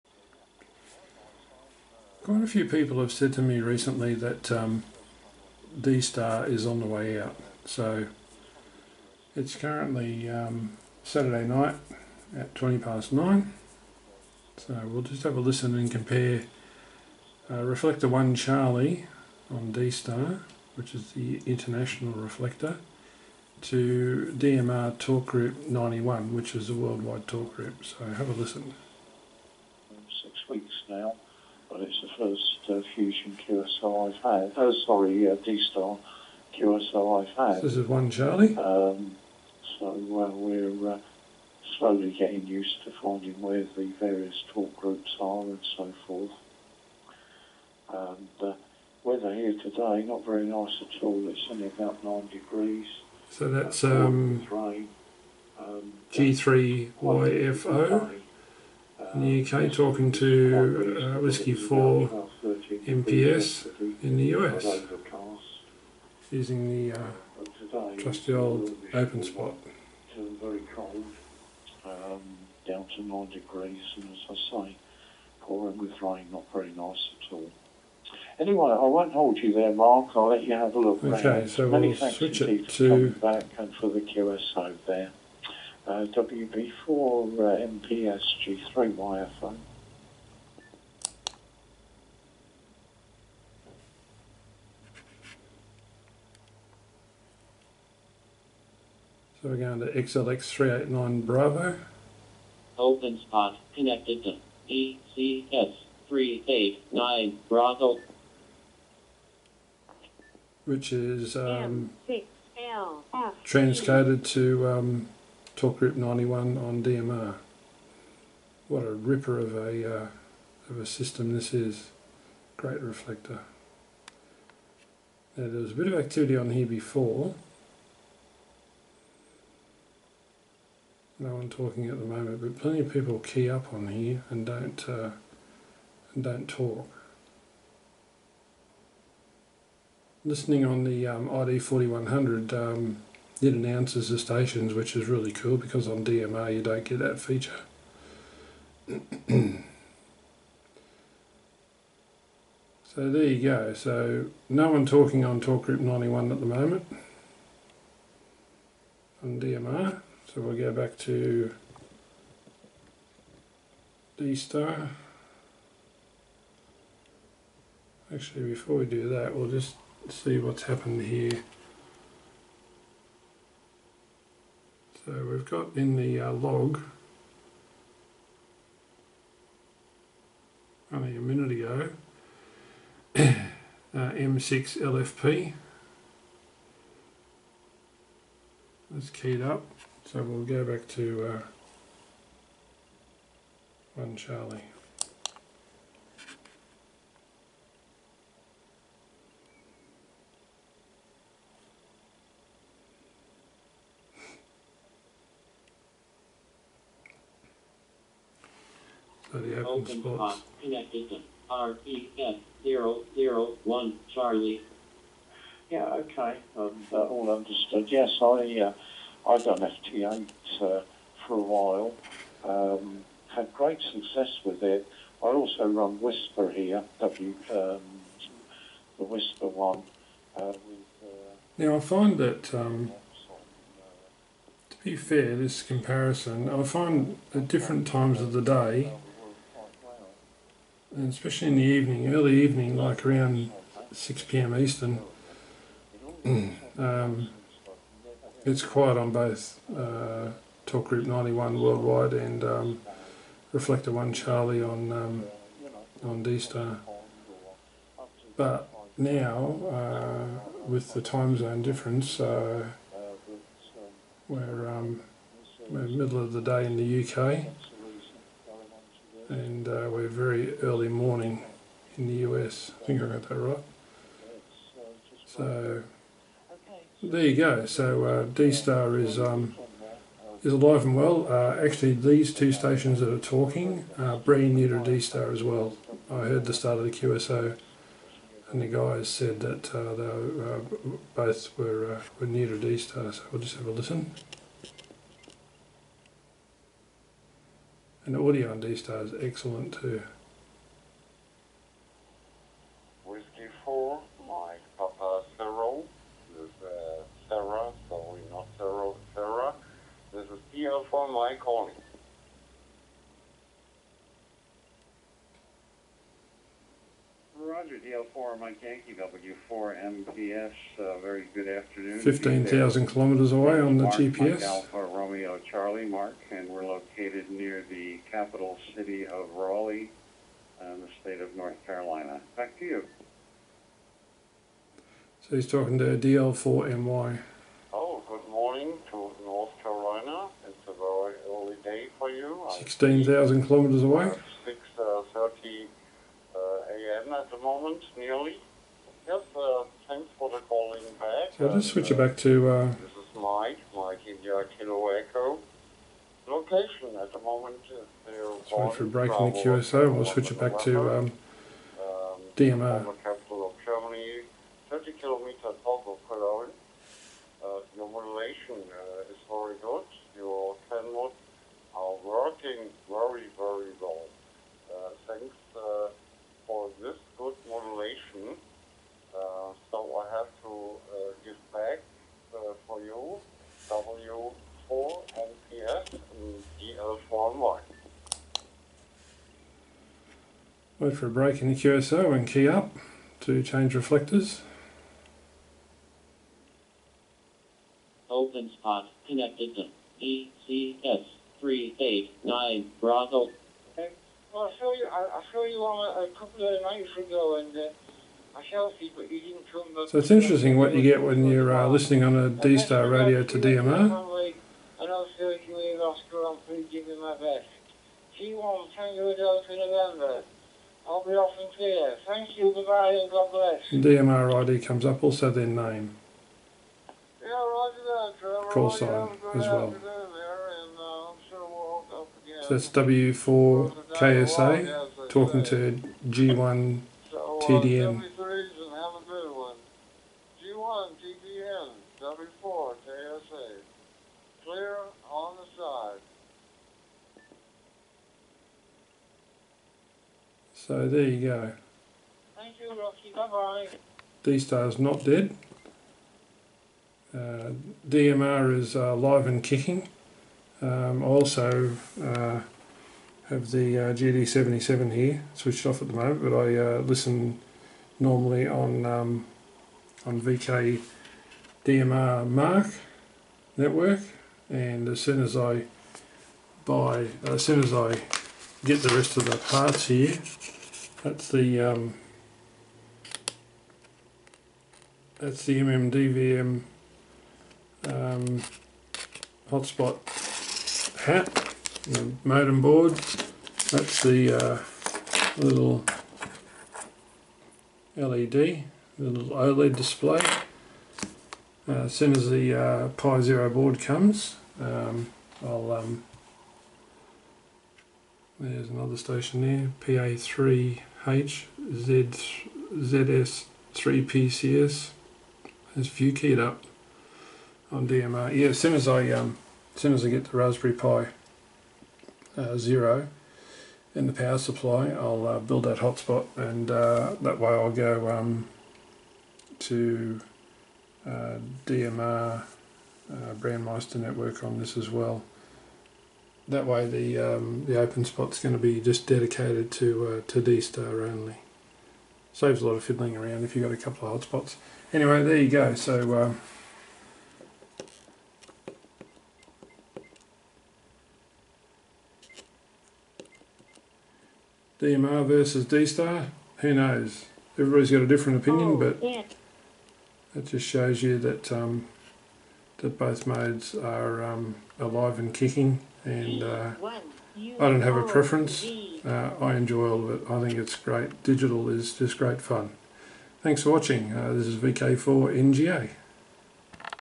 Is D Star dead? You decide. Dstar-DMR comparison
D Star to DMR comparison. In this video I compare D Star Ref001C to DMR Brandmeister Talk Group 91. There is talk on DMR that Dstar is on the way out. I disagree, but I don't have a preference either, I like all  DV modes! I'm using my ID-4100 on Dstar Ref001c and also the very cool transcoding reflector XLX389B linked to Brandmeister TG91.